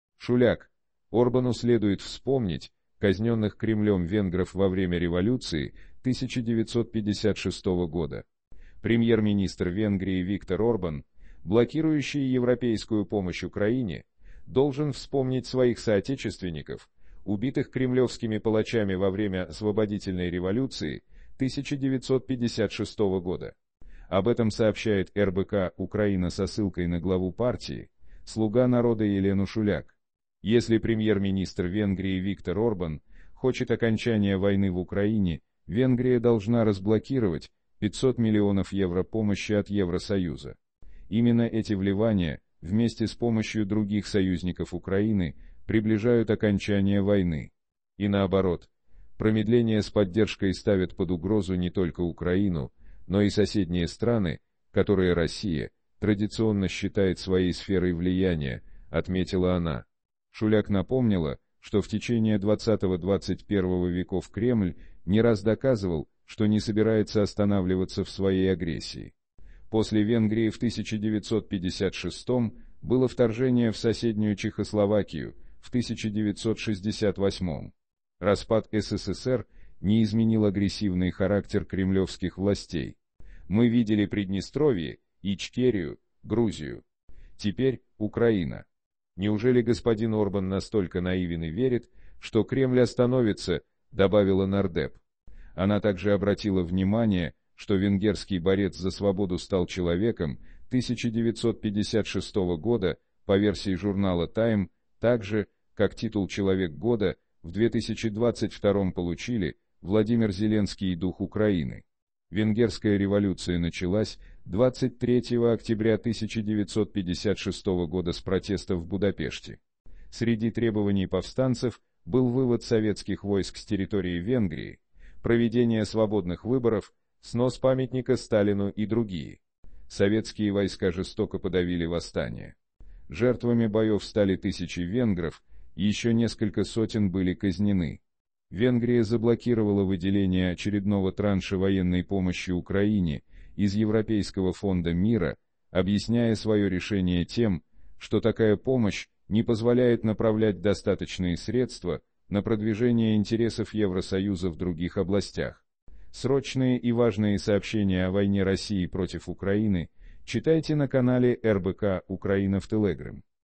Шуляк: Орбану следует вспомнить казненных Кремлем венгров во время революции 1956 года.
ЕленаШуляк Премьер-министр Венгрии Виктор Орбан, блокирующий европейскую помощь Украине, должен вспомнить своих соотечественников, убитых кремлевскими палачами во время освободительной революции 1956 года. Об этом сообщает РБК-Украина со ссылкой на главу партии "Слуга народа" Елену Шуляк. "Если премьер-министр Венгрии Виктор Орбан хочет окончания войны в Украине, Венгрия должна разблокировать 500 млн евро помощи от Евросоюза. Именно эти вливания вместе с помощью других союзников Украины приближают окончание